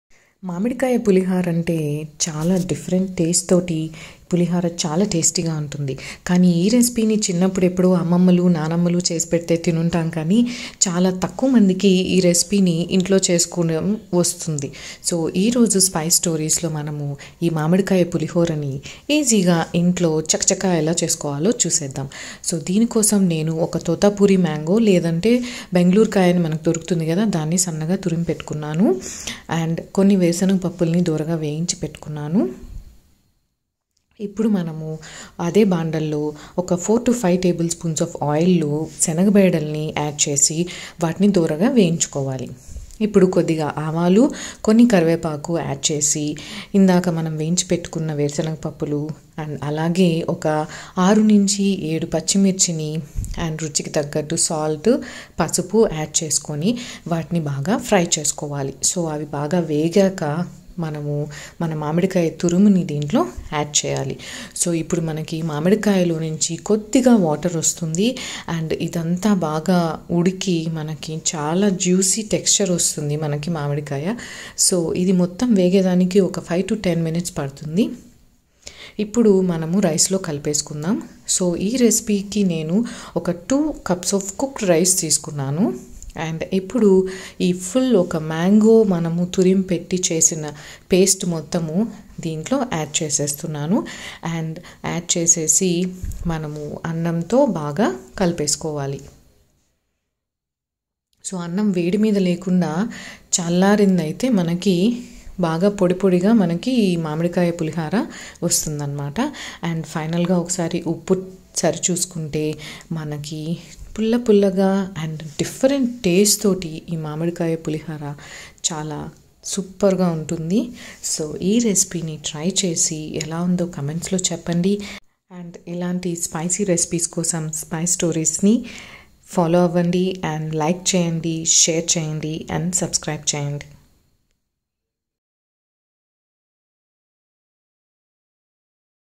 మామిడికాయ పులిహోర | Mamidikaya Pulihora Recipe | Village Food
Mango pulihora mamidikayapulihora recipe is a village summer speacial recipe . It Tastes Yum with a tangy and pungent mango taste .

Ingredients Required

Mango 1 grated
green chilly 4 to 6
salt as per taste
channa dal 1 tbsp
ground nut 1 tbsp
mustard 1 tbsp
curry leaves
turmeric powdere 1tsp